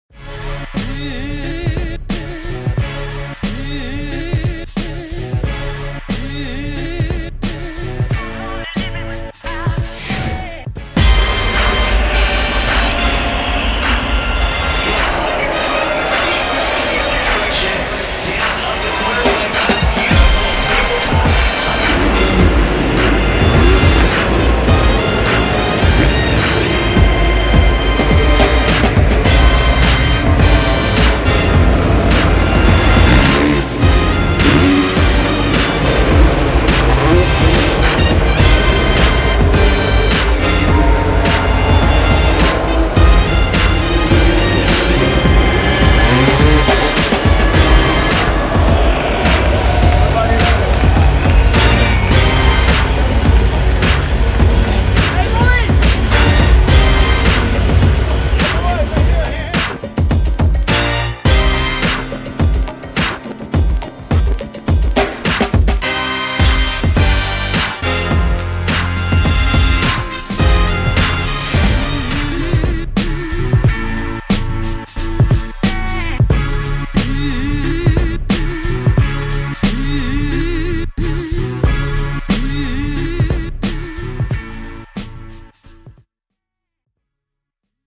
Virtual Reality: Day 2 Highlights | X Games Minneapolis 2017
The best moments in Virtual Reality from day two of X Games Minneapolis 2017!